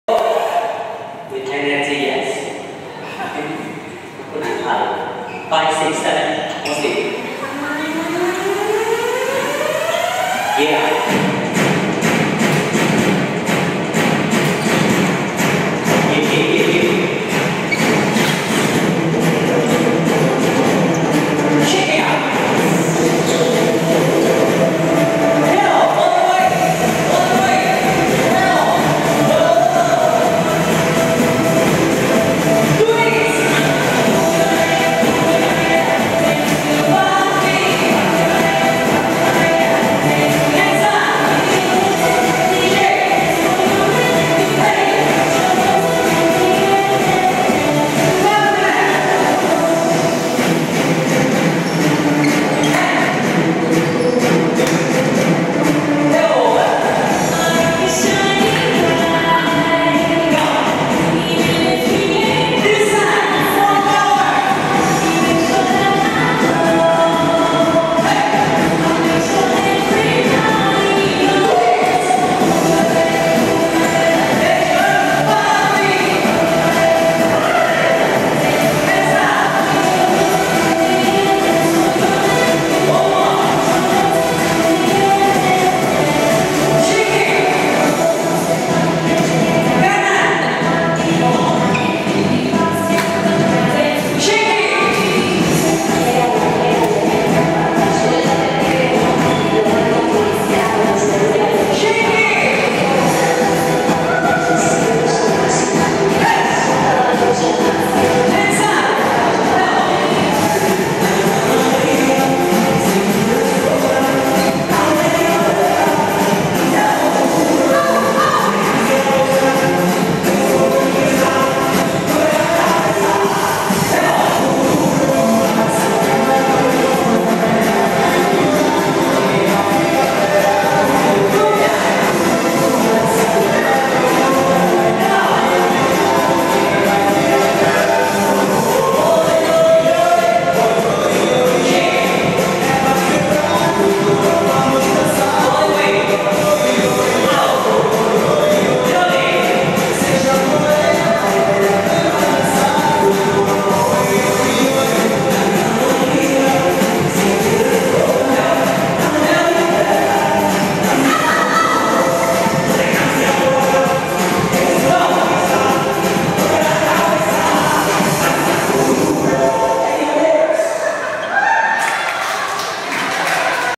CF MY - 2012 DNA Valentine's Day Special by Baby G & Vidal
Celebrity Fitness, One Utama Malaysia. Gillian Lee Dance Class